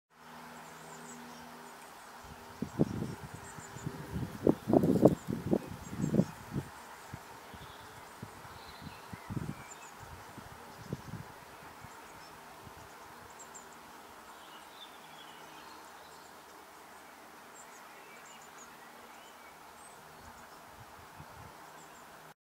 Spider Nest Breakout
Unknown species spiderlets breaking out of their nest cucoon.
Norfolk, England.